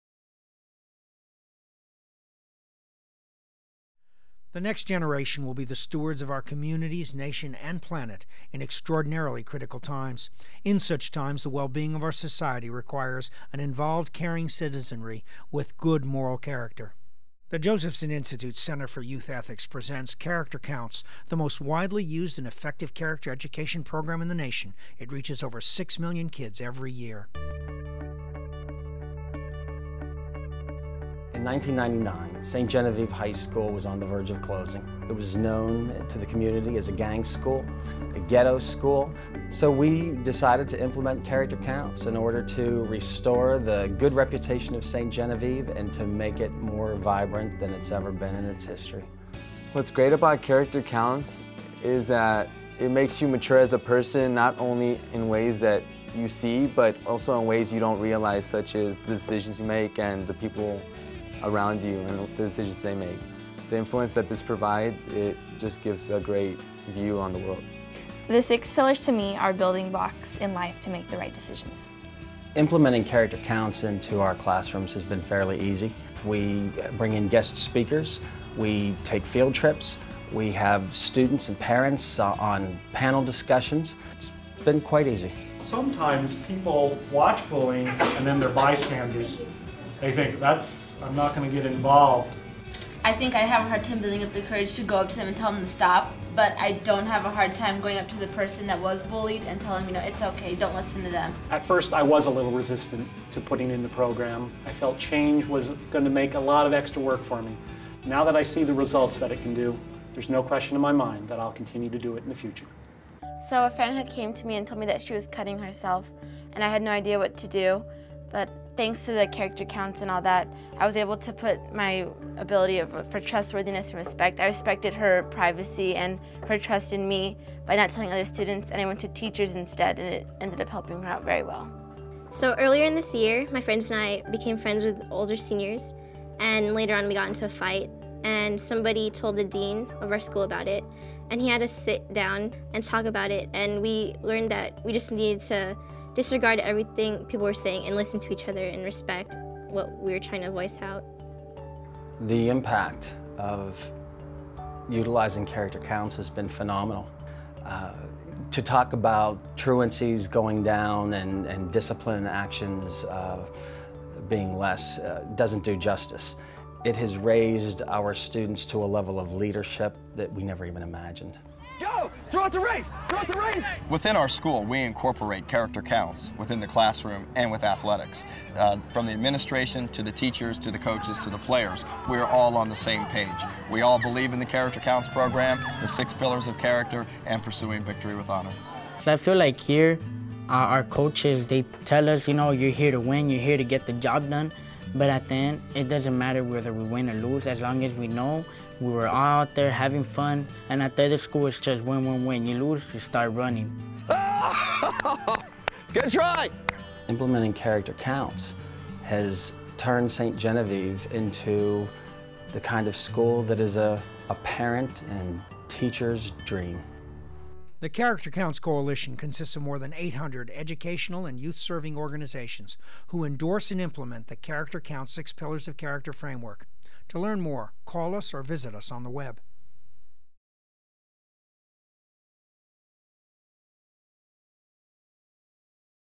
Character Education Success in High School
This video profiles a high school in California and shows the benefits of using CHARACTER COUNTS!, the most popular character education program in the United States.